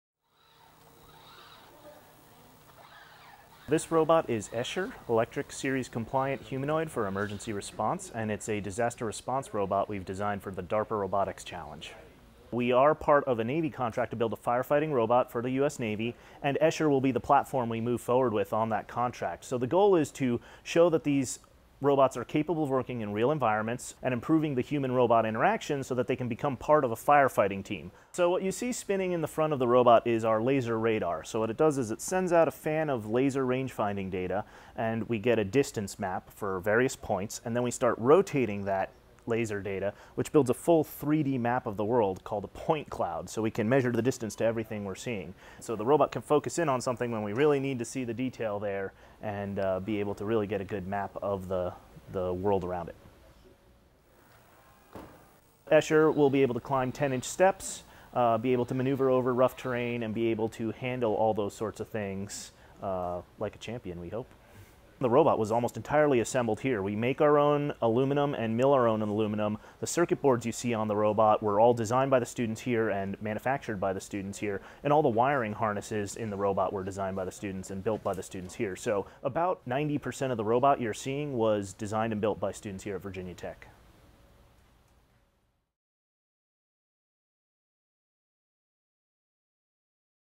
Meet ESCHER, the robot that rescues - Virginia Tech
Virginia Tech's latest student-made robot is ESCHER, and he's ready for competition. In the DARPA Robotics Challenge finale this week, he'll go up against other robots from around the world to complete a series of rescue and recovery tasks. A total of $3.5 million is at stake, with $1 million for second place and $500,000 for third place.